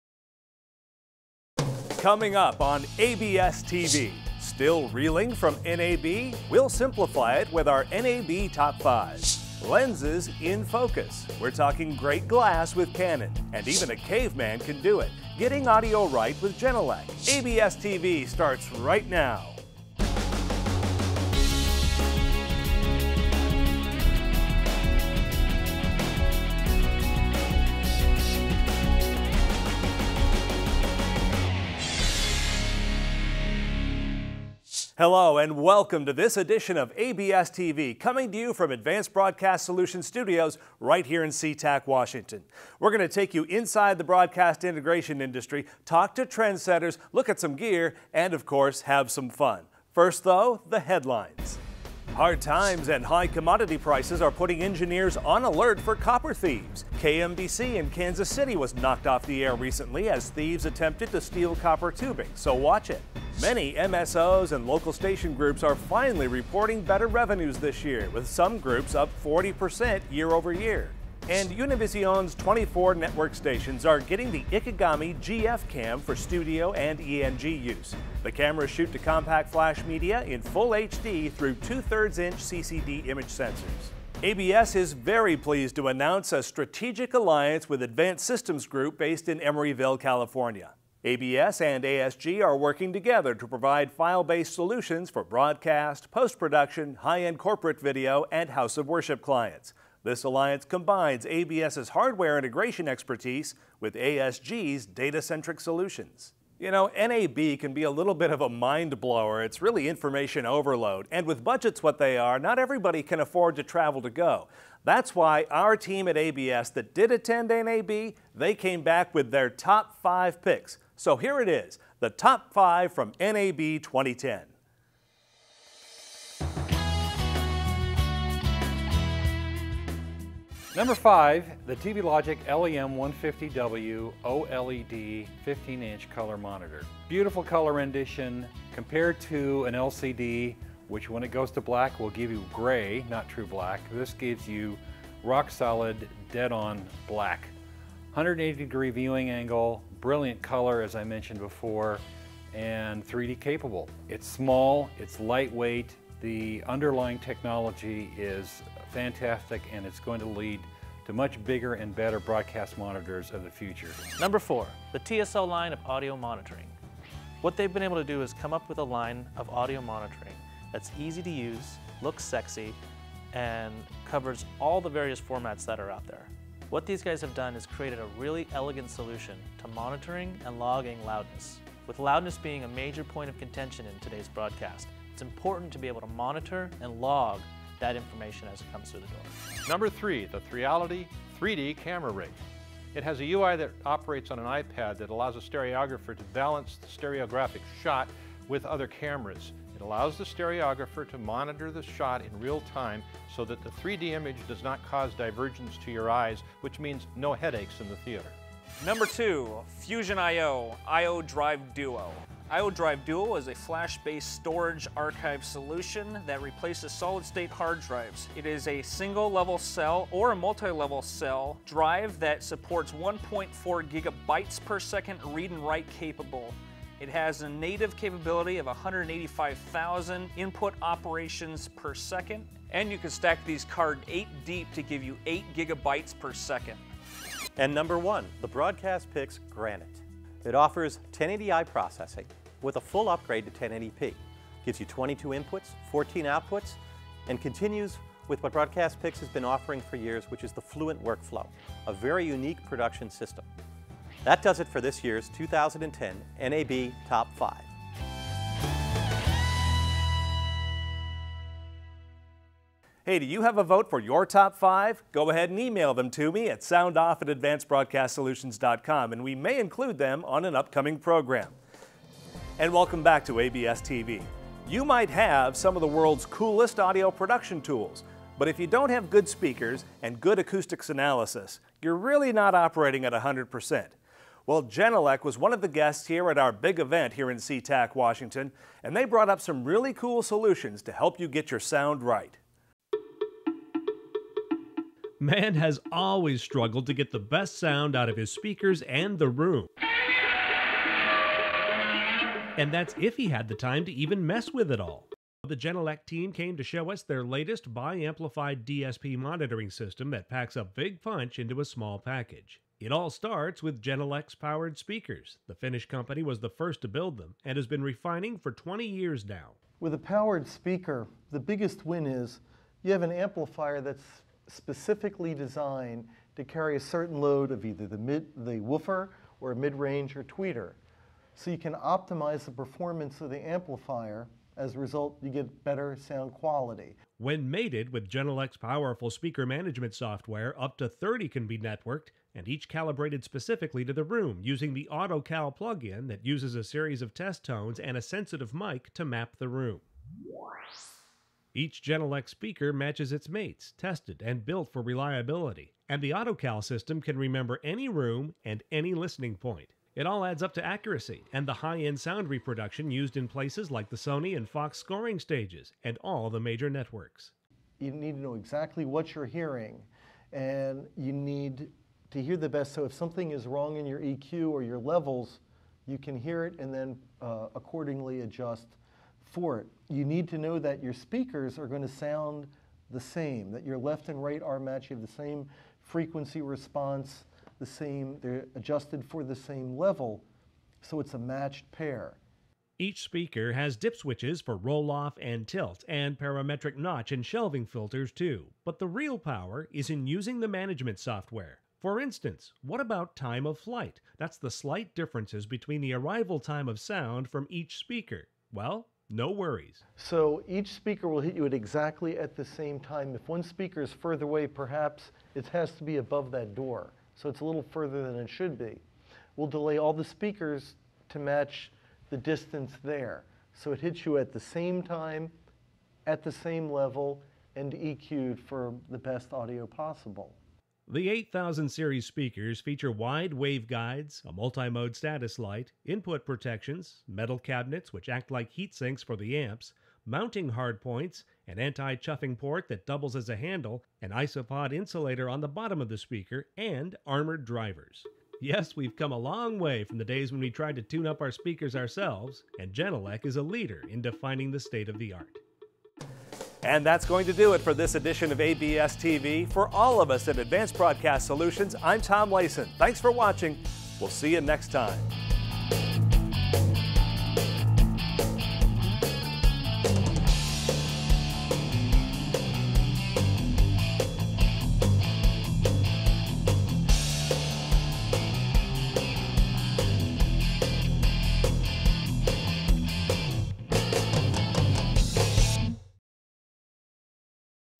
The latest edition of ABSTV
This time we get the ABS team's NAB top 5 and learn more about products from Genelec. The interview with Canon in the second segment can be viewed on this Youtube channel as a separate item.